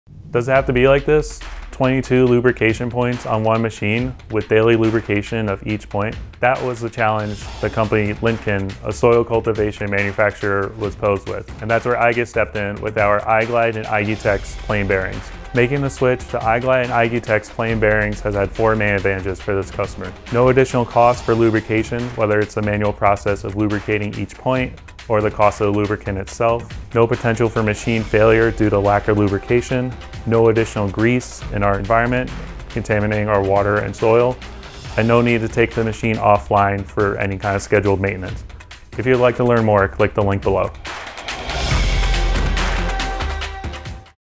Lemken Application Solution iglide® & igutex® bearings
Lemken, a manufacturer of soil cultivation equipment, approached igus with the task of eliminating the need to lubricate 22 lubrication points, per machine, per day. With dry-running iglide® and igutex® plain bearings, this was achieved while also eliminating the possibility of grease entering the environment.